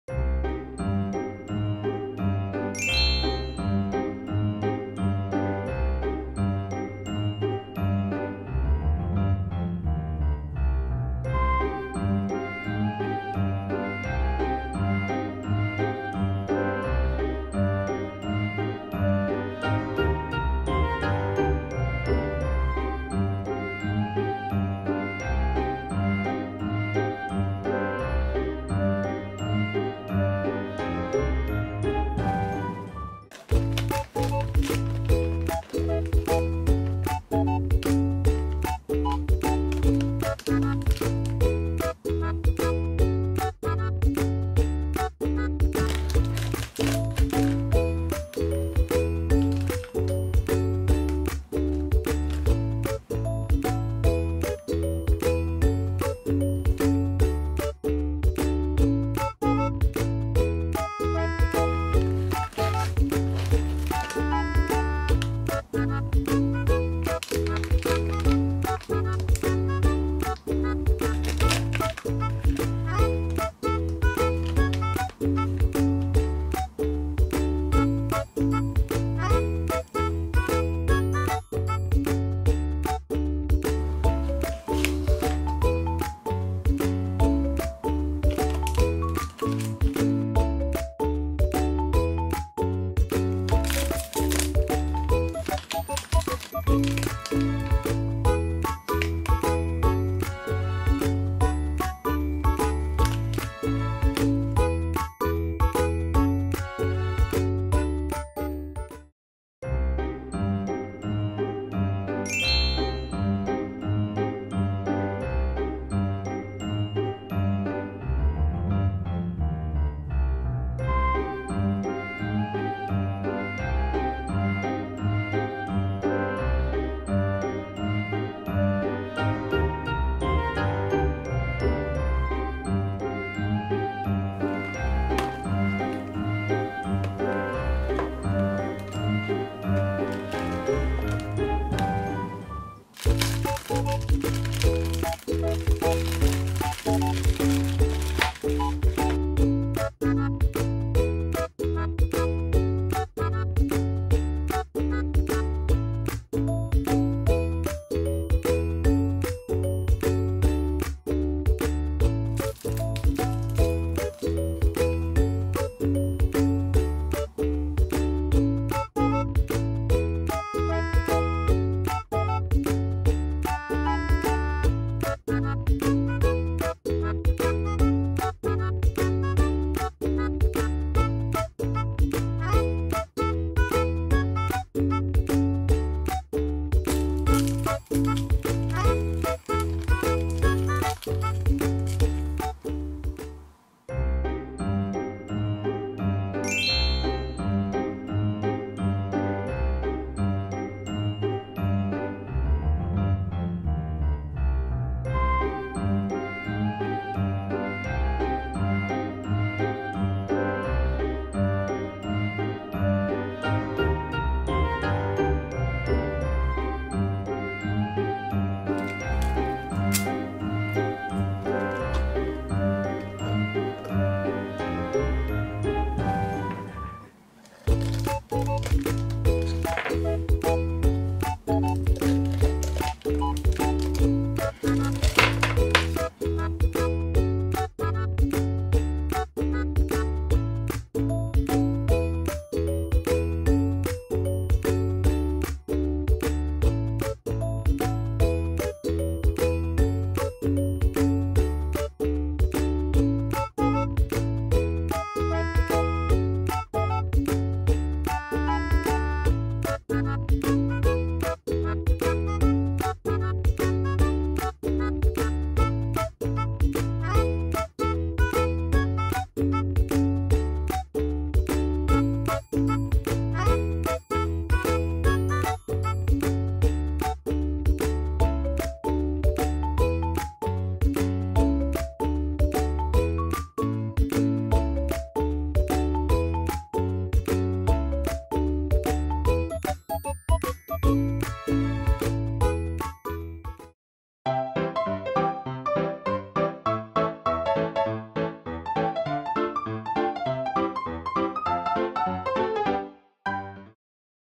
すみっコぐらし ハロウィンお菓子を開封♪Halloween 角落生物 fromegg
ショッピングセンターですみっこぐらしのハロウィンのおかしを発見したのでいくつか買ってきました♪
イラストがどれも可愛すぎるのでハロウィンのイベントにもおすすめですよ〜♪(*´∀｀*)